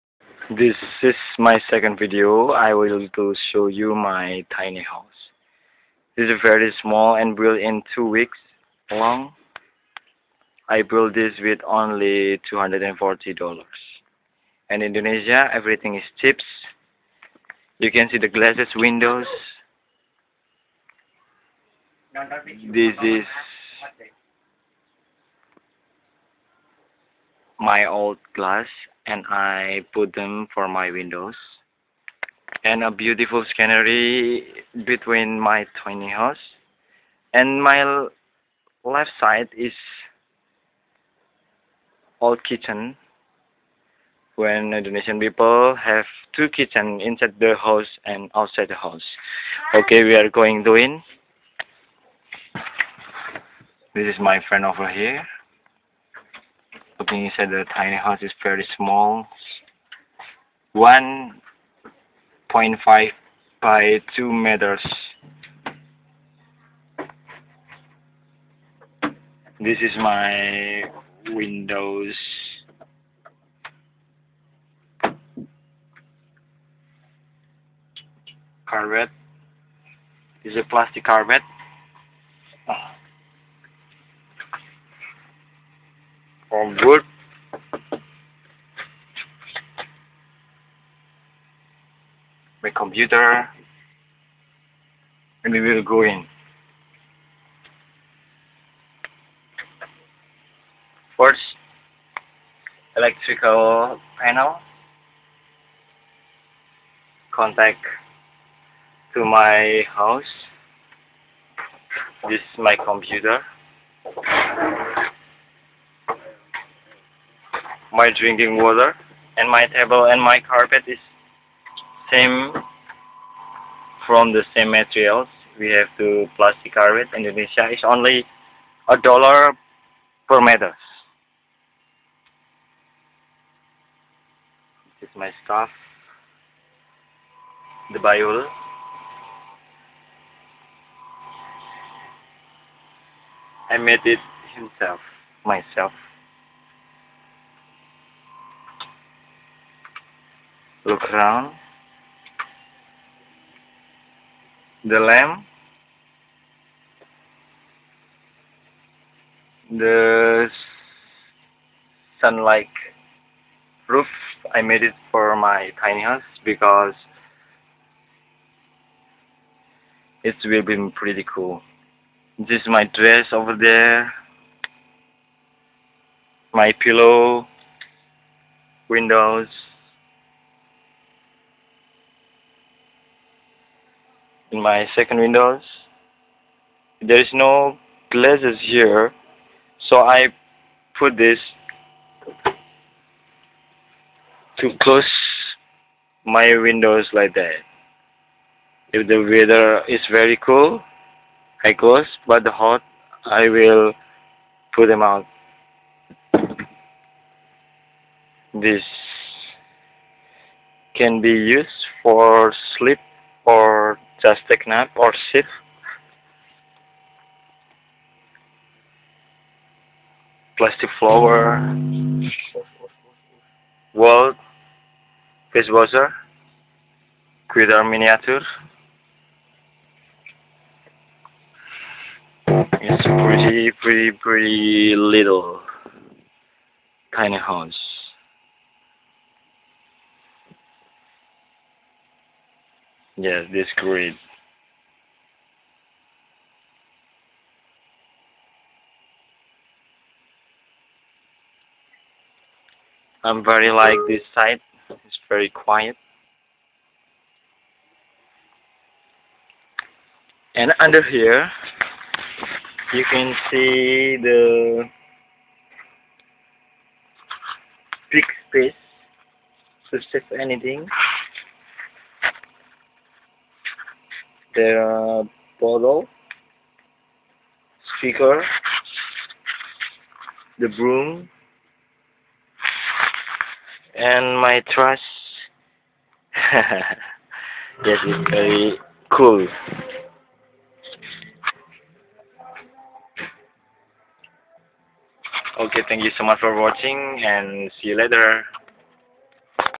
real $56 tiny house indonesia
i will show you a room that amazing to use and stay. the tiny house was built in 2 weeks, and this is it